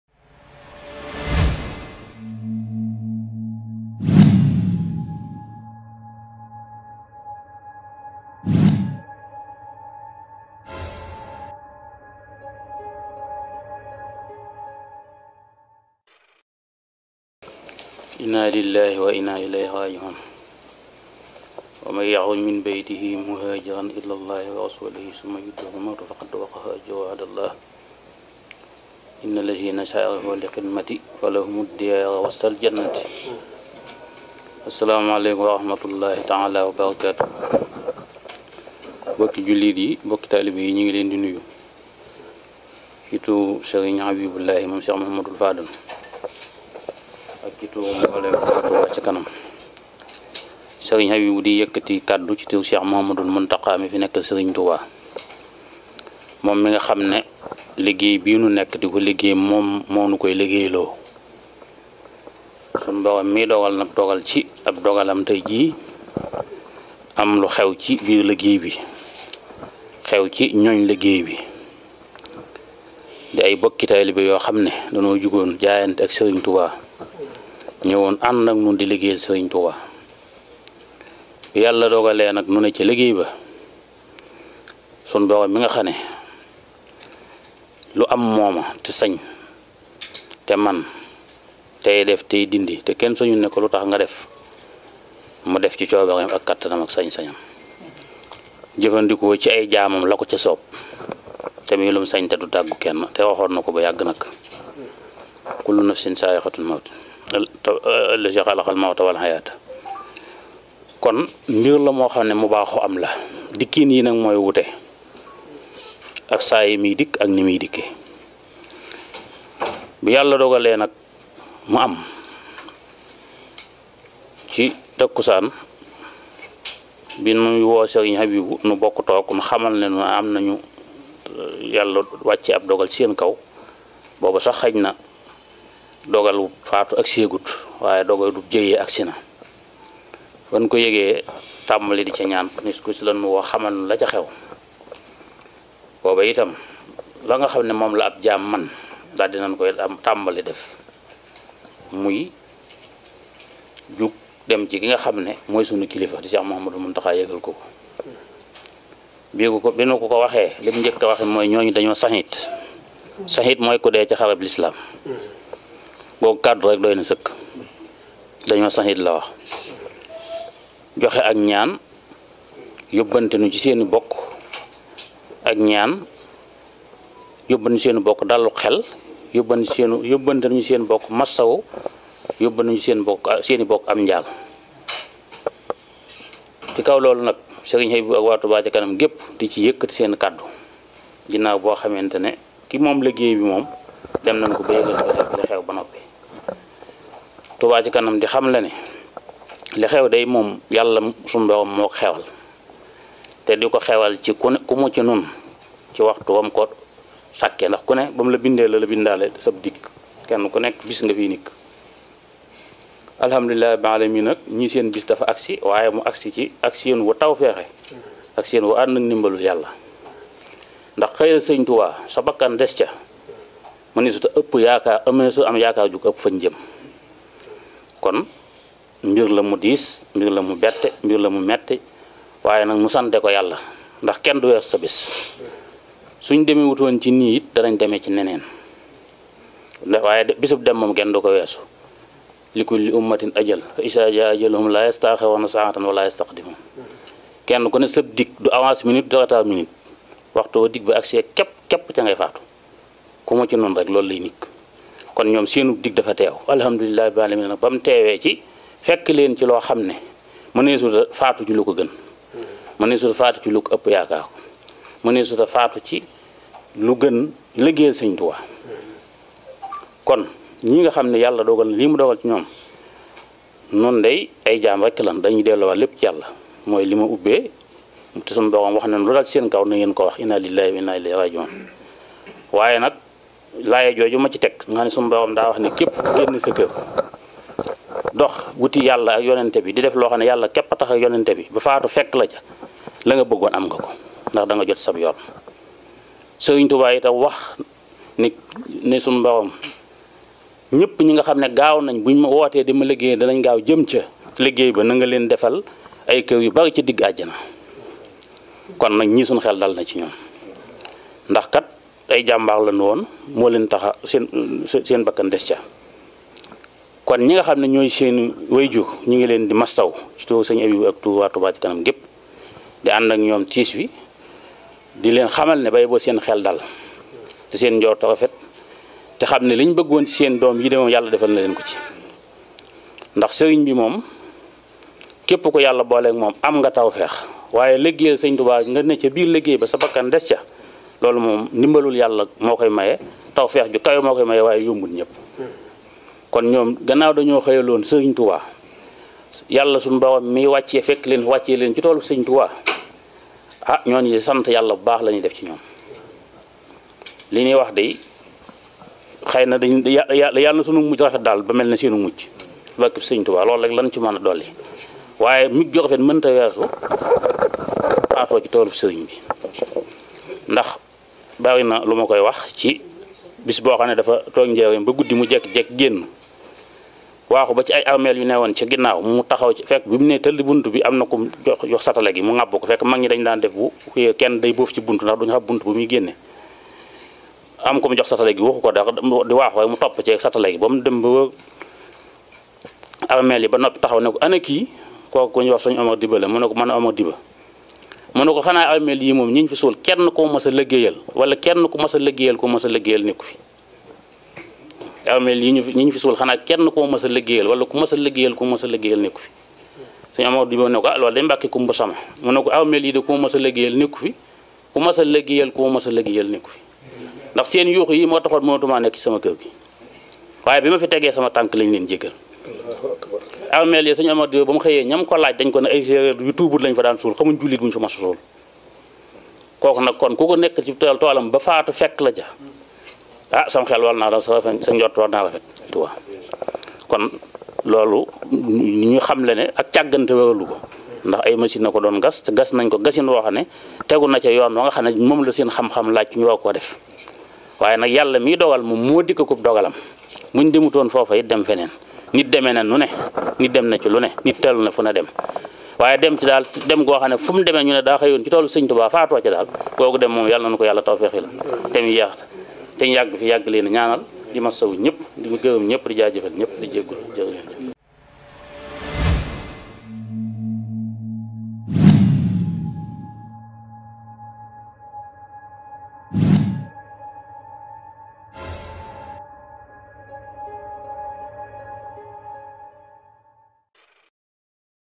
Touba Ca Kanam - Décés par accident de 2 personnes : S. Habibou Mbacke présente ses condoléances
Touba Ca Kanam - Décés par accident de 2 personnes : S. Habibou Mbacke présente ses condoléances au nom du Khalif général des Mourides#